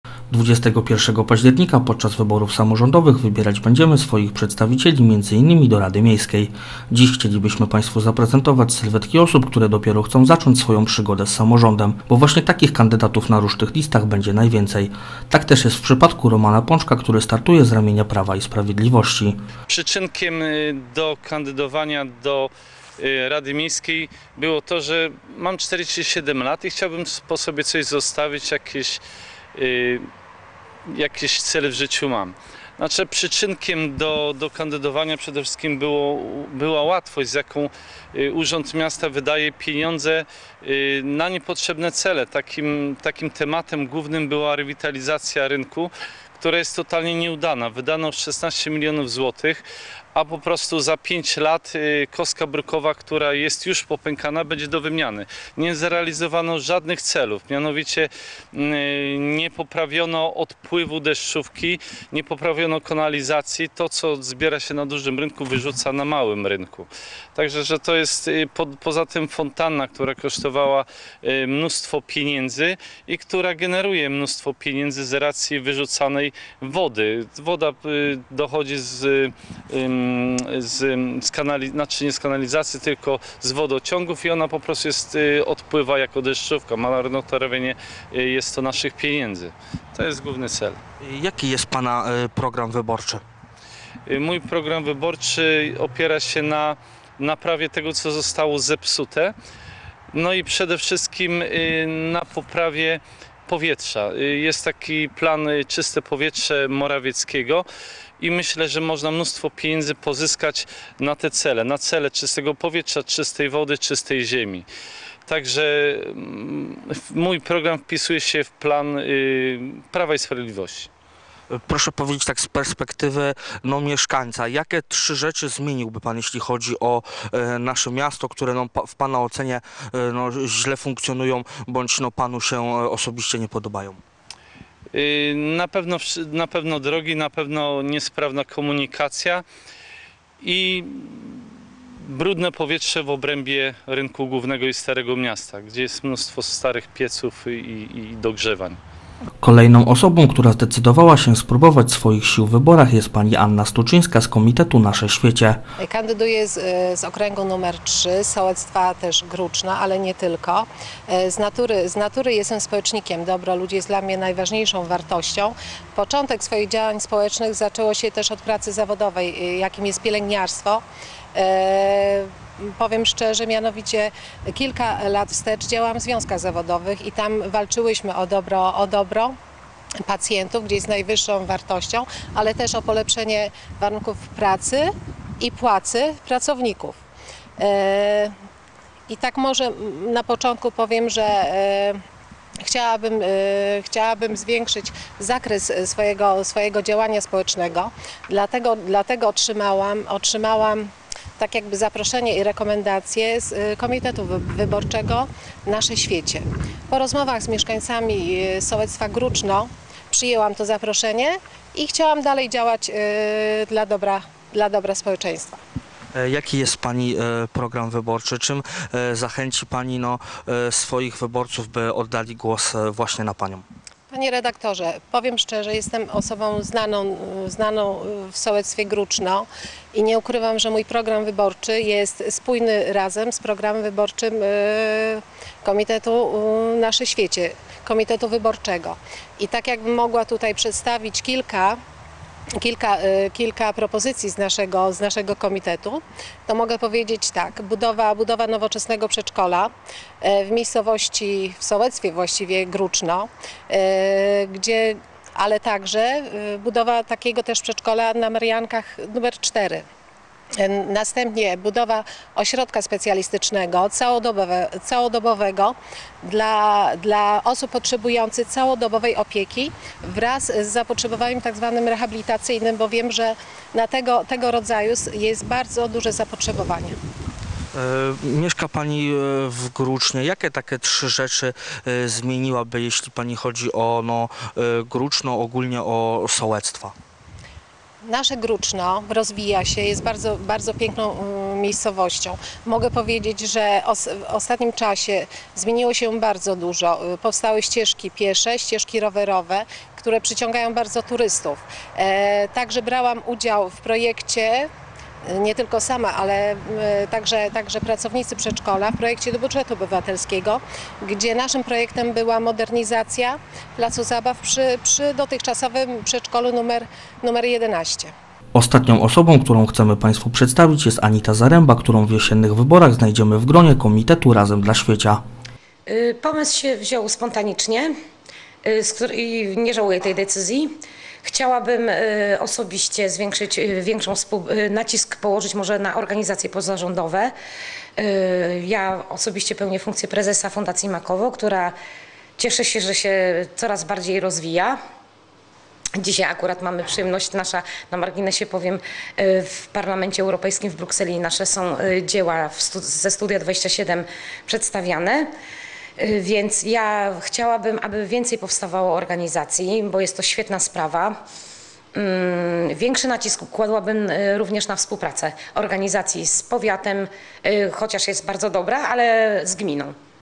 TV CSW: Walczą o głosy mieszkańców (cz. 1)
Do wyborów samorządowych została już tylko ostatnia prosta. O względy mieszkańców walczą nie tylko doświadczeni samorządowcy,  ale również osoby, które dopiero chcą zacząć swoją przygodę z lokalną polityką. Wśród nich jest Roman Pączek (PiS), Anna Stuczyńska (Nasze Świecie) oraz Anita Zaremba (Razem dla Świecia).